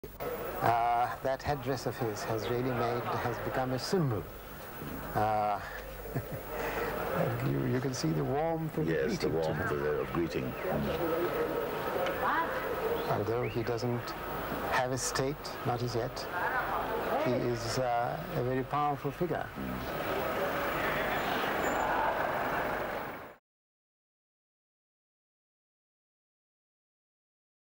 Yasser Arafat and Indira Gandhi in Delhi
That headdress of his has become a symbol. You can see the warmth with which Indira Gandhi and Yasser Arafat are greeting each other. Although he does not have a state not as yet but he is a very powerful figure.

Mohammed Yasser Abdel Rahman Abdel Raouf Arafat al-Qudwa al-Husseini, popularly known as Yasser Arafat or by his kunya Abu Ammar, was a Palestinian political leader. He was Chairman of the Palestine Liberation Organization from 1969 to 2004 and President of the Palestinian National Authority from 1994 to 2004.